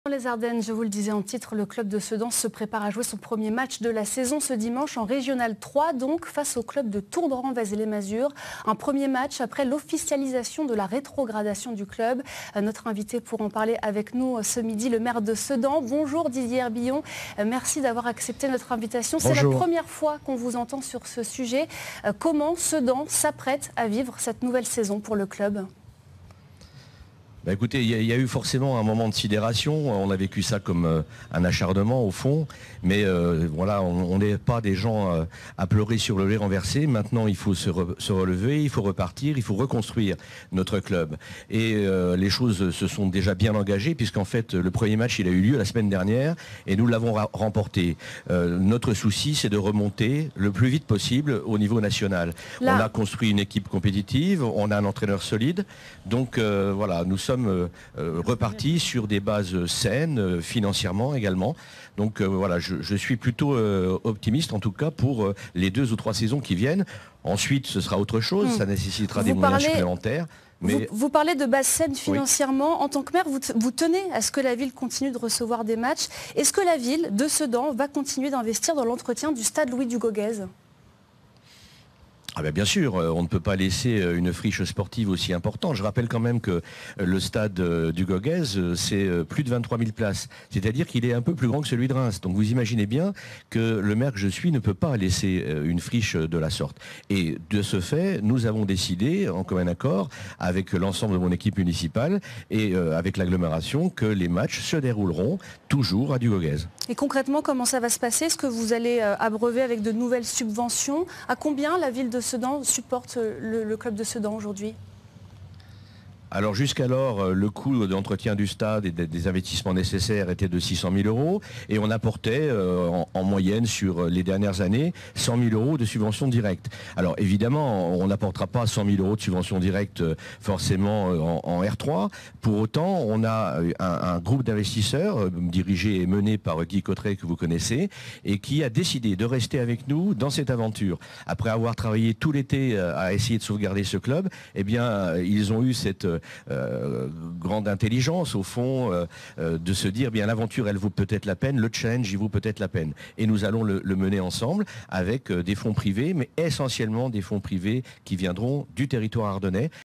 Didier HERBILLON sur France 3 le 6 septembre 2023
Sedan repart en R3 et jouera bien à Dugauguez